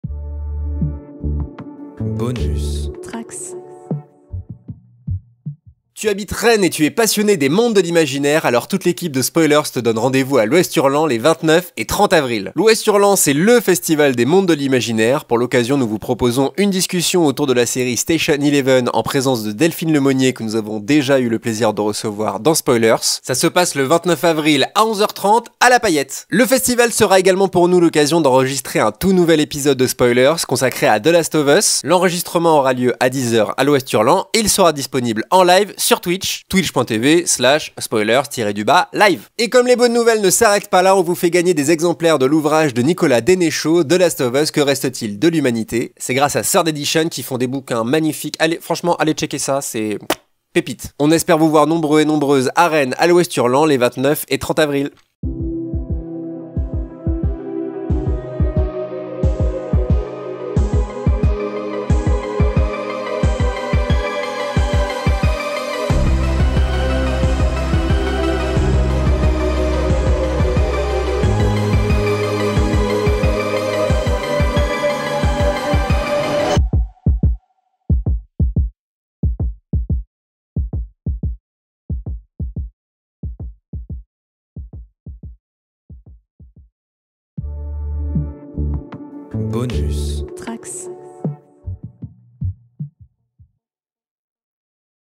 29 & 30 Avril · Festival l'Ouest Hurlant x SPOILERS, à Rennes !
Cette année, nous avons le plaisir de vous inviter à deux rendez-vous en mode post-apo en sein de la deuxième édition du Festival l'Ouest Hurlant !

• Samedi 29 avril à 11h30 à la Paillette Théâtre, nous vous proposerons un dialogue autour de STATION ELEVEN avec Delphine Lemonnier-Texier, enseignante-chercheuse à l'Université Rennes 2 ! L'occasion de revenir sur cette très belle série de Patrick Somerville (The Leftovers, Maniac).

• Dimanche 30 avril à 10h, nous serons sur Twitch en direct du Festival pour une émission consacrée à la série THE LAST OF US où nous reviendrons entre autre sur les adaptations de jeux vidéo en série.

On espère vous voir nombreux et nombreuses pour cette deuxième édition du Festival l'Ouest Hurlant !